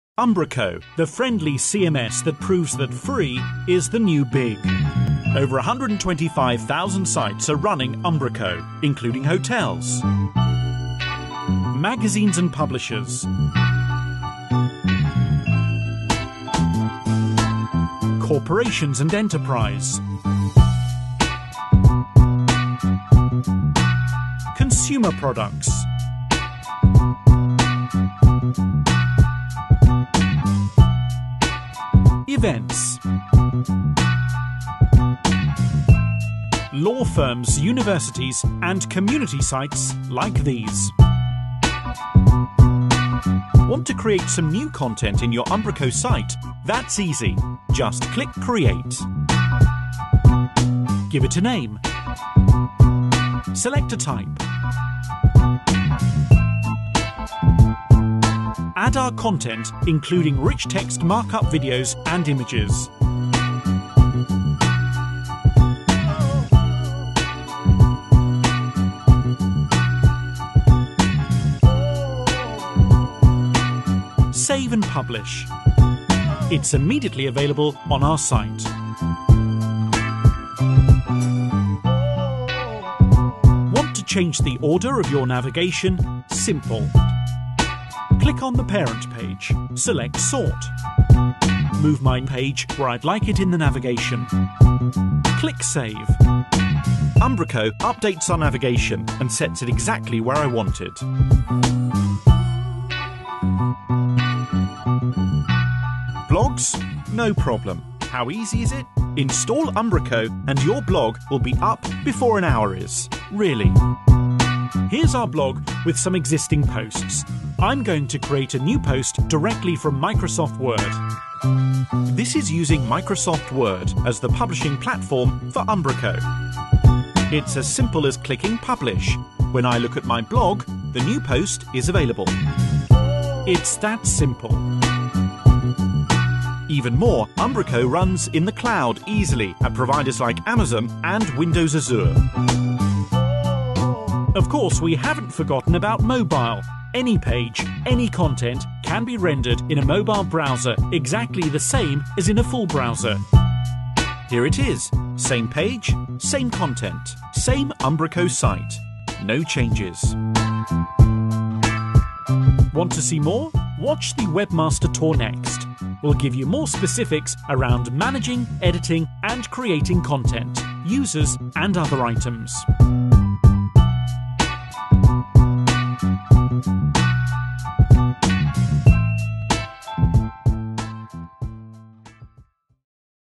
What is Umbraco CMS?
An introductory tour of the Open Source .NET based CMS Umbraco.